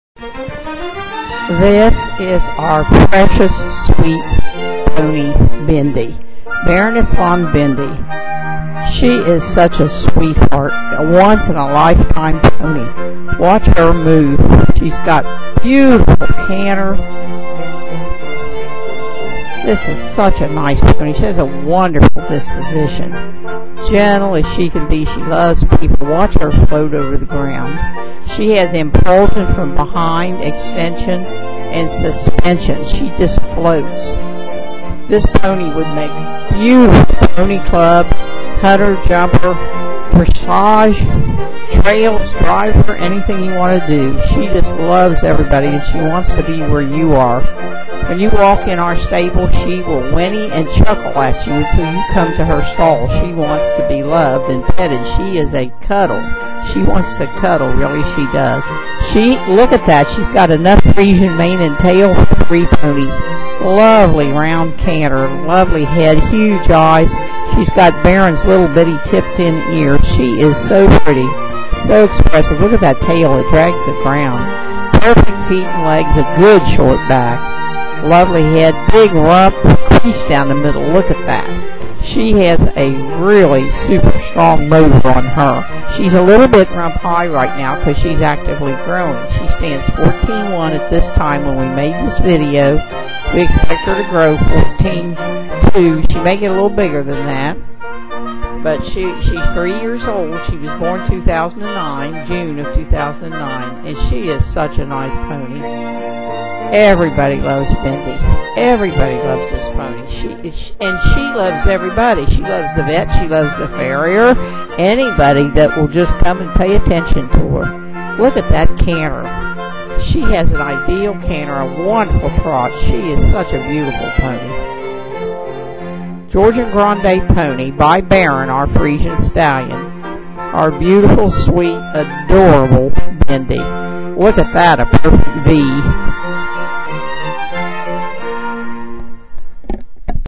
BARONESS VON BINDI -SOLD (more available!)
FLYING W FARMS BARONESS VON BINDI -SOLD
More like her available - both geldings and mares!

Bay  Friesian Georgian Grande mare (pony) foaled  June 1, 2009
Sire Flying W Farms Baron von Kolderveen (Friesian)
mother is Flying W Farms Venus (Saddlebred/gypsy draft pony)
estimated mature height 14.2 hands
THIS IS A ONCE IN A LIFETIME PONY!
You have, (we assure you) never  met a pony quite like our sweet Bindi; She seems to understand english! She is so good, and she totally adores people! She whinnies to everyone she sees when they walk in the stable. If you are looking at another horse she continues to make little "chuckle" noises until you walk over to her. She loves to cuddle, her disposition is amazing; you need to meet her to believe it. George says Bindi would come in the house and sit on the sofa with us if we let her. Robin, our daughter adores this sweet pony; Our son says he never in his life saw a pony so sweet and friendly;

Bindi has a profuse Friesian mane and tail that nearly drags the ground. She is strong and sturdy with excellent bone, perfect legs and feet; She is so sweet and pretty, has her sire's tiny little tipped in ears, and huge eyes; Bindi has wonderful balance and her movement is incredible, she seems to float at the trot. Her walk is brisk and her canter so smooth! She will be a very comfortable ride;

Bindi stands 14.1 now at three years of age. She is a bit rump high right now because she is actively growing. (normal growth pattern of equine)

No video can convey Bindi's disposition; We would love to have you come meet her;  Bindi  would be ideal for pony club, jumping,  driving, dressage, trails, anything you want to do; She is so very willing and loves to be with you. Price $5000